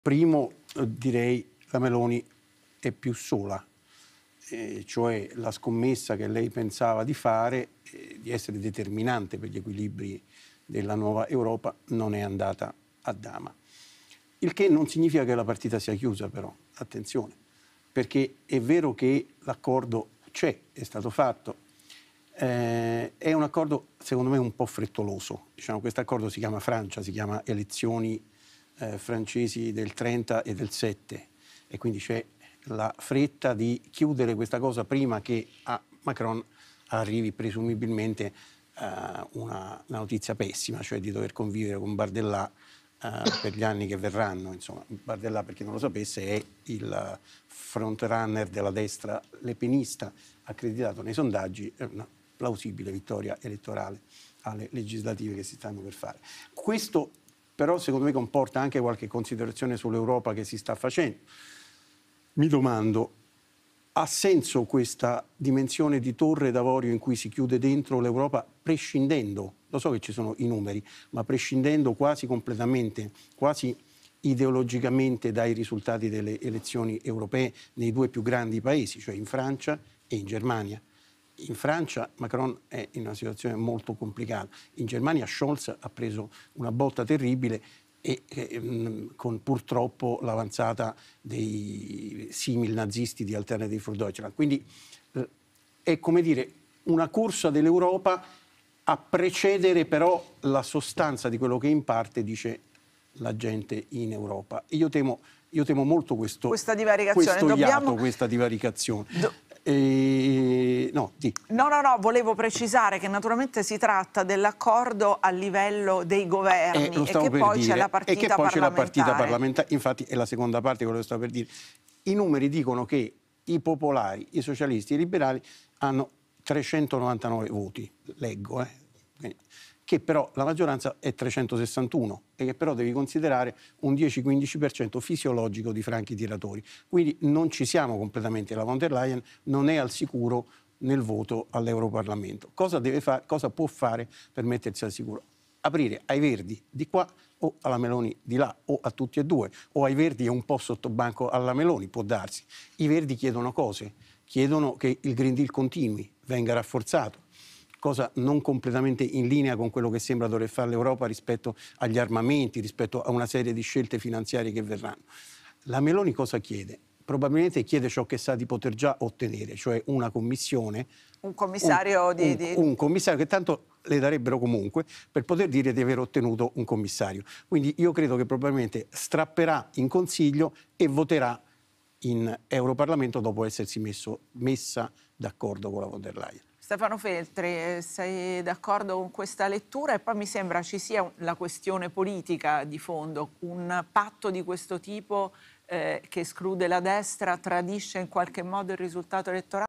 Nomine UE, Buccini: "Meloni più sola ma Von der Leyen non è al sicuro nel voto in Parlamento"
Il commento di Goffredo Buccini sulle nomine europee e lo smacco a Giorgia Meloni